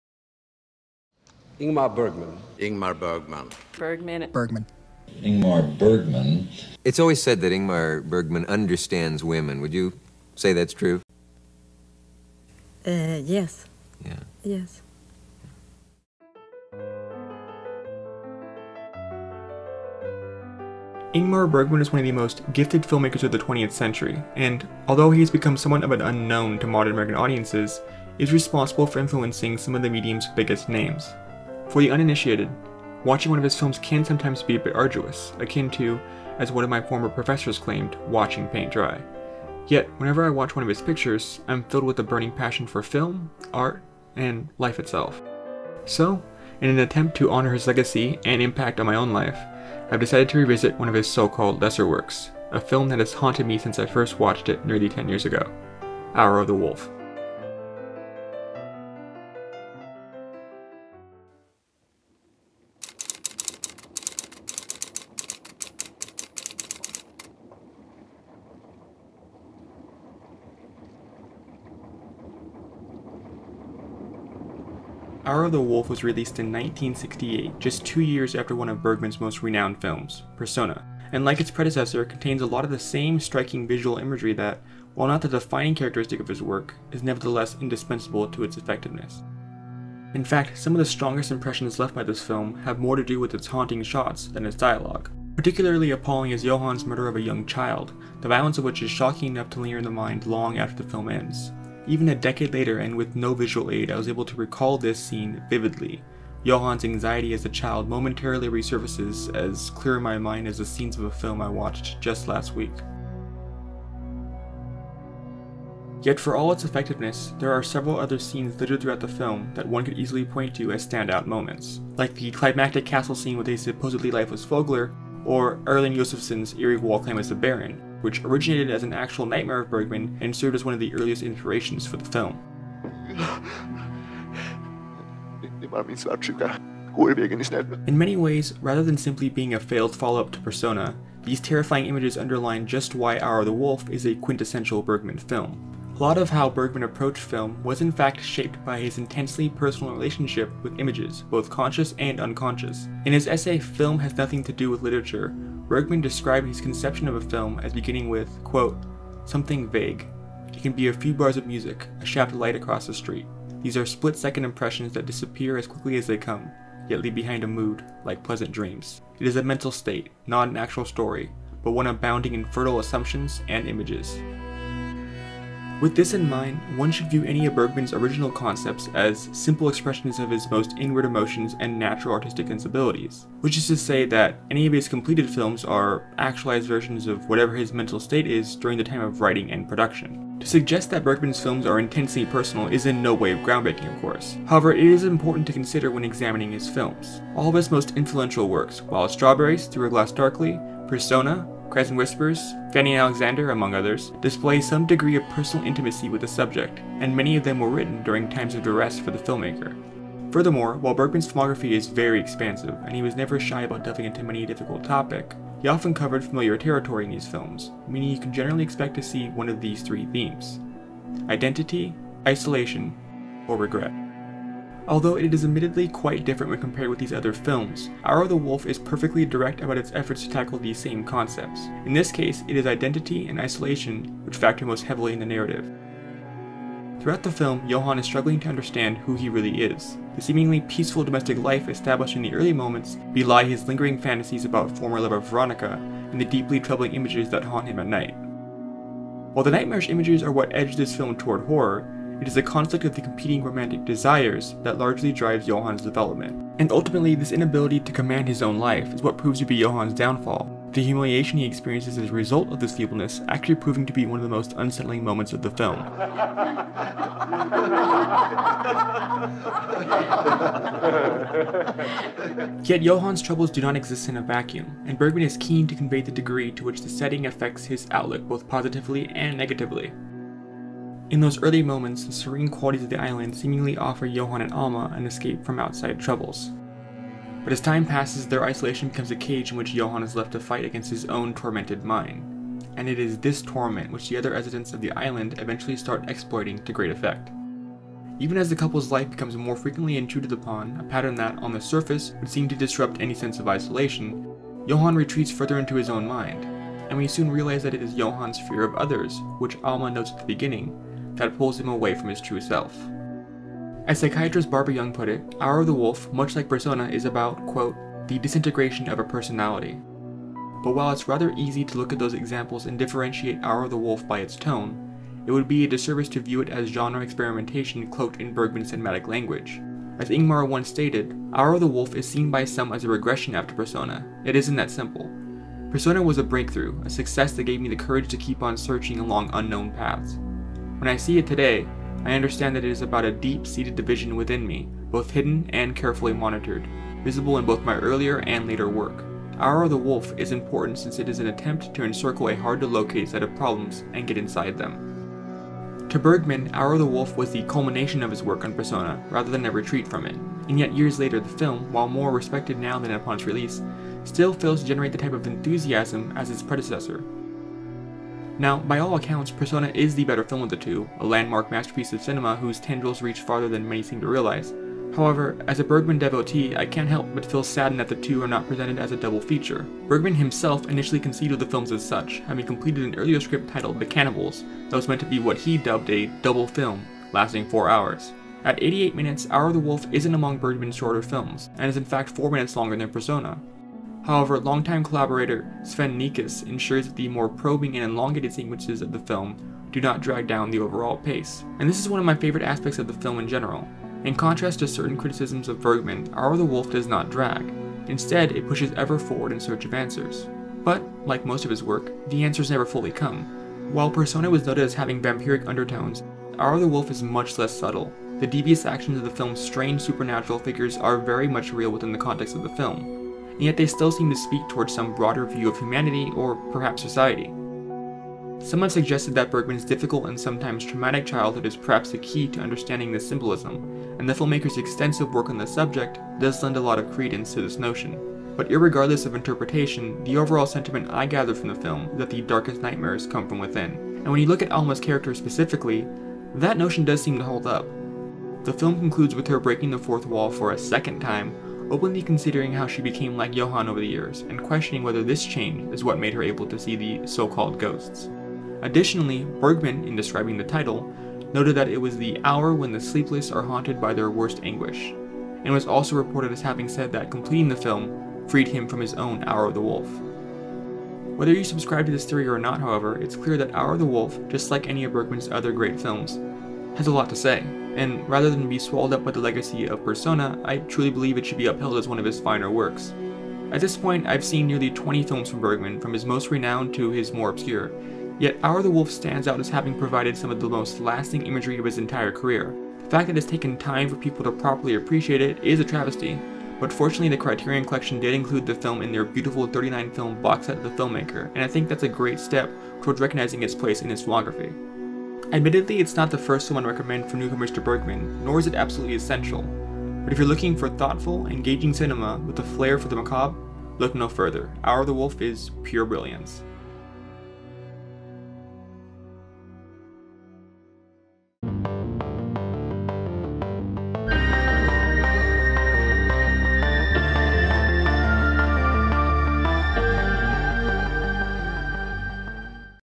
Examining Bergman: The Brilliance of Hour of the Wolf (1968)
Ingmar Bergman was one of the most gifted filmmakers of the 20th century, but has become somewhat of an unknown to modern American audiences. In an attempt to honor his legacy and impact on my own life, I've decided to revisit one of his so-called lesser works: Hour of the Wolf (1968)